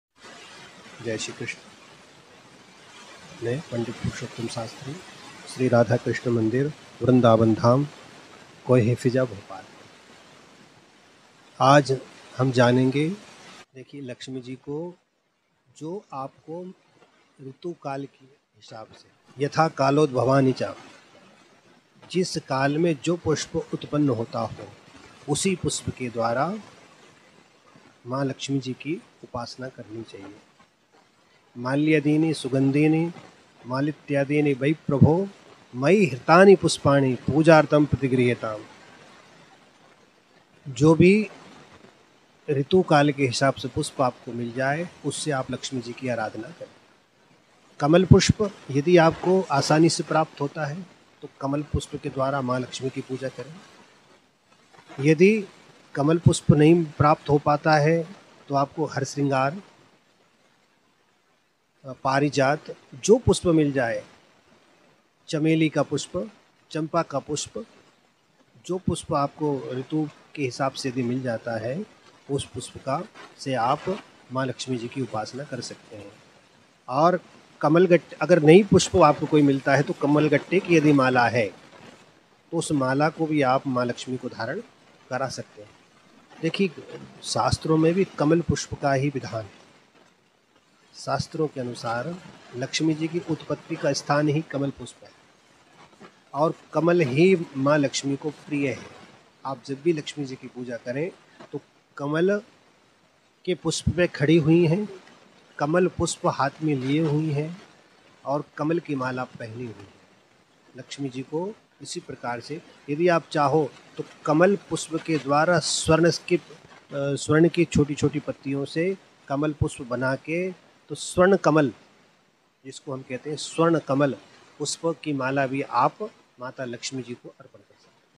Diwali 2023 : दिवाली के दिन मां लक्ष्मी को चढ़ाया जाता है ये फूल, हर तरफ से होगी धन वर्षा!
Bhopal News : देशभर में दिवाली की बड़े ही धूमधाम से तैयारी चल रही है. ऐसे में भक्तगण धन की देवी माता लक्ष्मी को प्रसन्न करने के लिए हम कई तरह के उपाय करते हैं और चाहते हैं कि मां प्रसन्‍न होकर अपनी कृपा बरसाएं.
Bhopal News: Preparations for Diwali are going on with great pomp across the country. In such a situation, the devotees take various measures to please Goddess Lakshmi, the goddess of wealth, and want the Mother to be happy and shower her blessings.